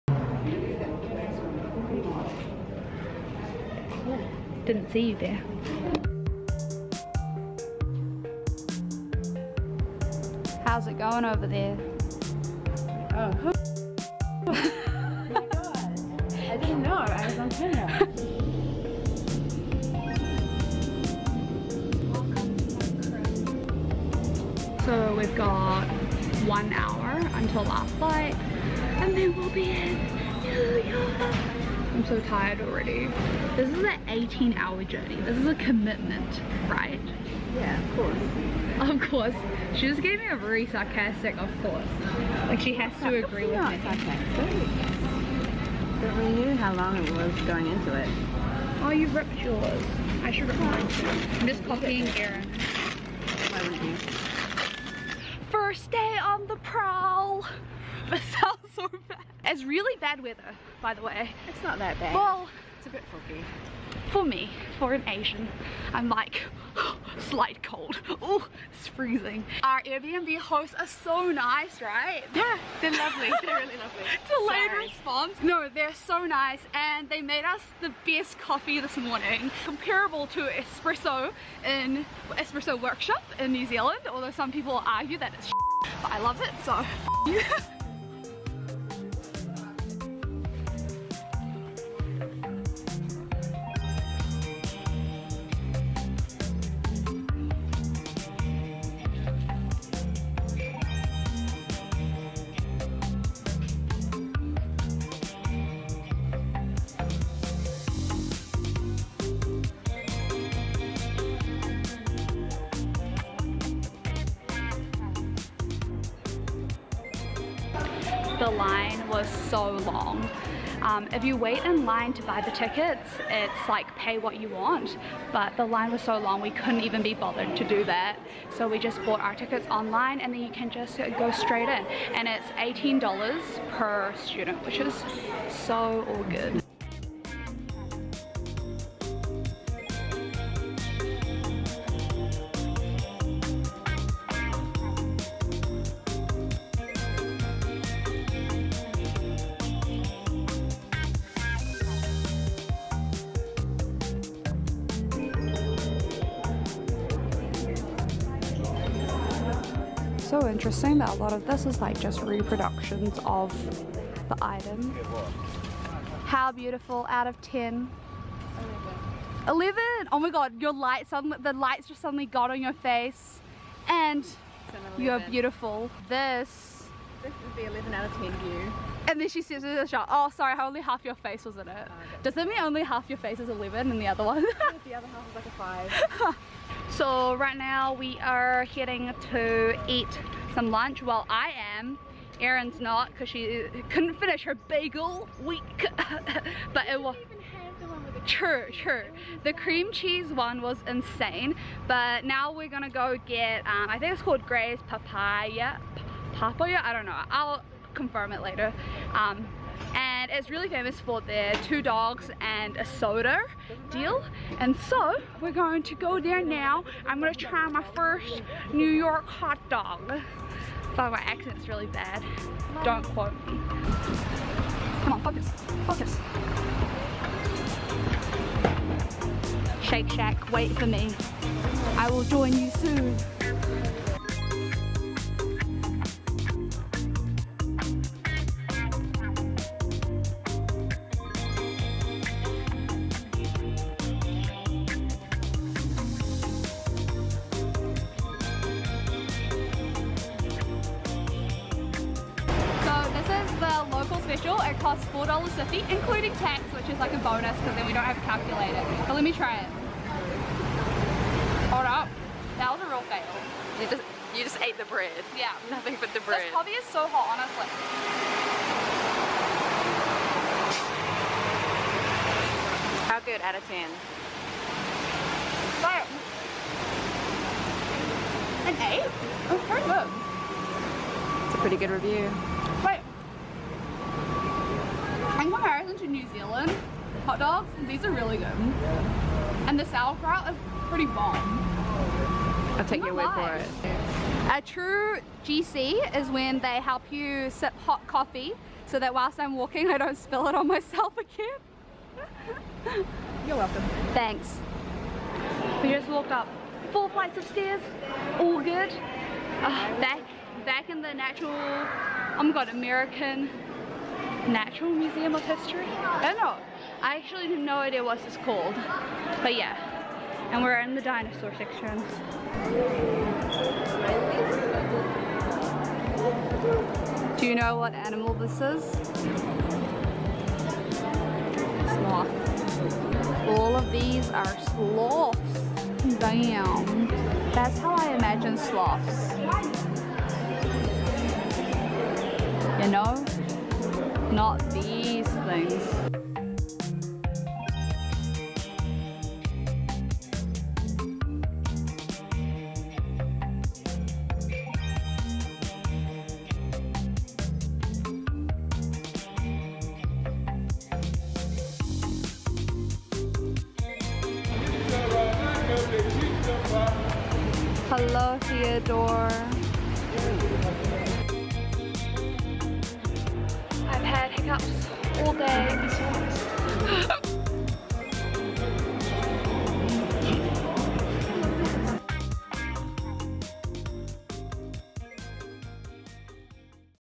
New York City For First Timers [VLOG 1] ~ Joyce Wong
Watch a Kiwi girl go to NYC for the first time!

List of places that I went to:
- The Bagel Store Original Rainbow Bagel
- American Museum of Natural History
- Gray's Papaya